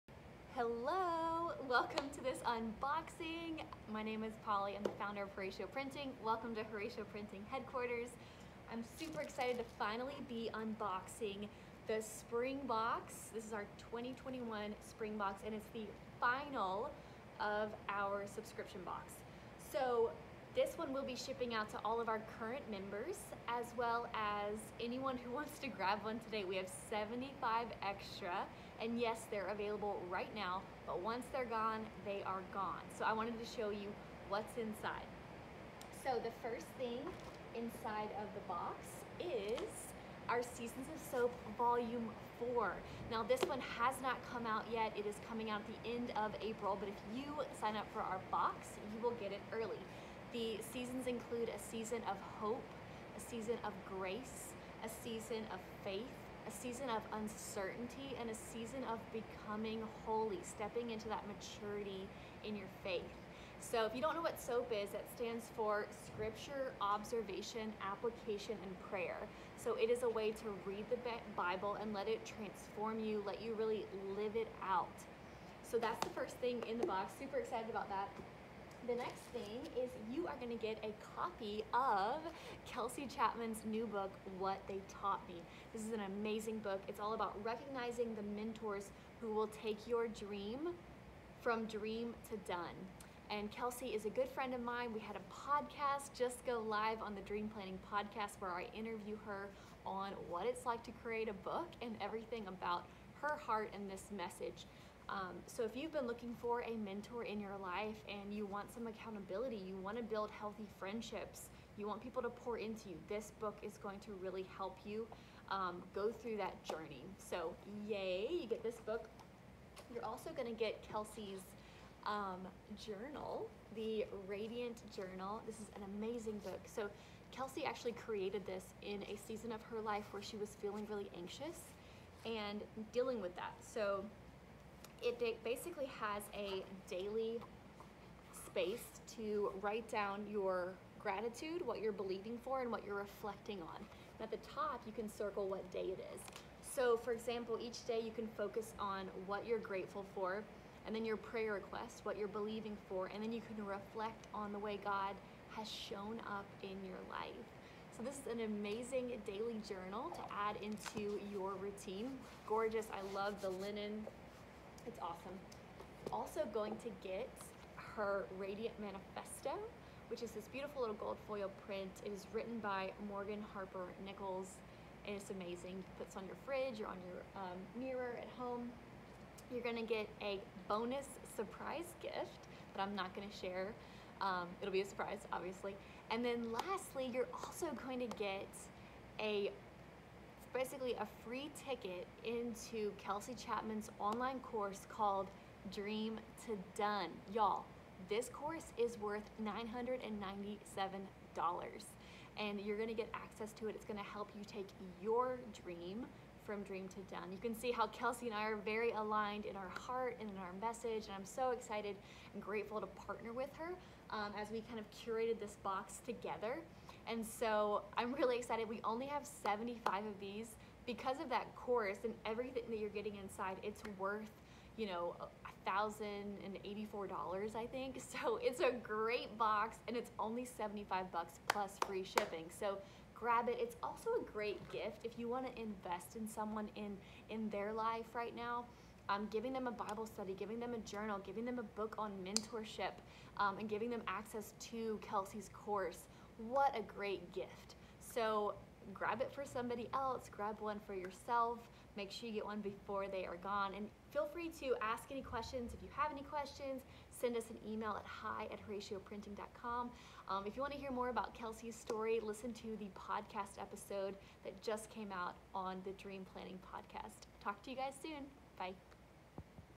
Unboxing the 2021 Spring Box from Horacio Printing
The Spring Box Includes:
*Seasons of Soap Bible Study, Volume 4
*Radiant Journal
*What They Taught Me, by Kelsey Chapman
*Gold Foil Print
*Digital Access to Dream To Done (valued at $997!)
*Bonus Gift!